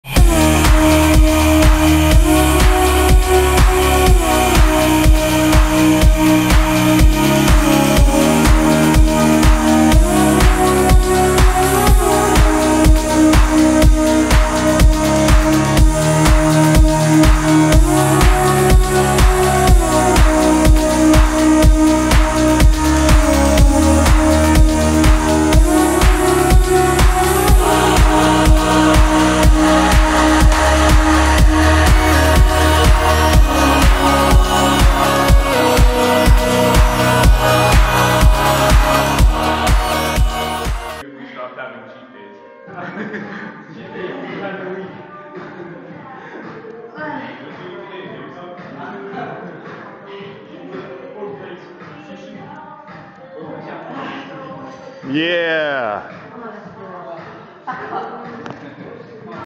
Acceleration Performance training - Blast Straps Bi/Tri superset
Nikki blasting her guns using the blast straps at Acceleration Performance